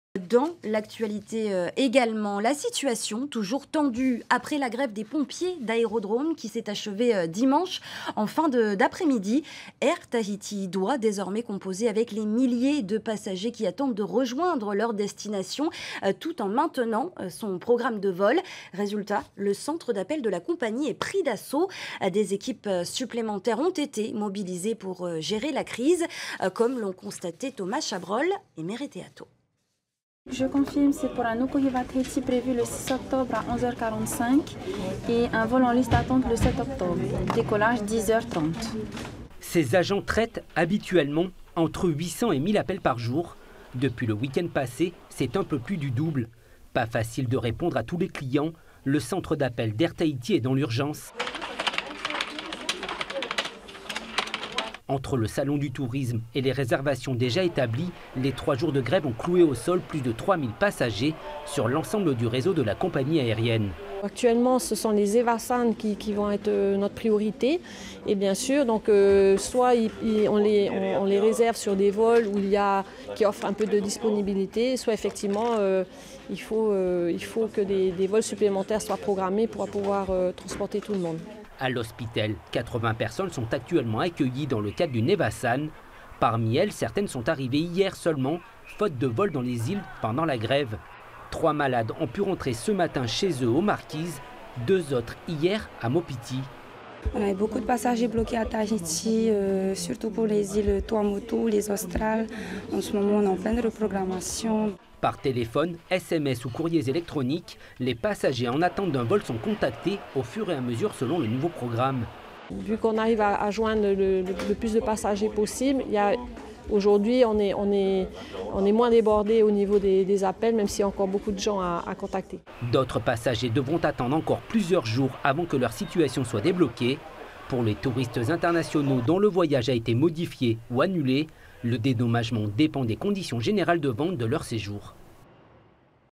Le centre d'appels d'Air Tahiti pris d'assaut après la grève
Les dix avions d’Air Tahiti  multiplient les rotations pour assurer le transport des milliers de passagers qui ont vu leur vol décalé ou annulé pendant les 3 jours de grève. Le centre d’appel de la compagnie déborde de coups de fils. Et des équipes en renfort ont été sollicitées. Reportage.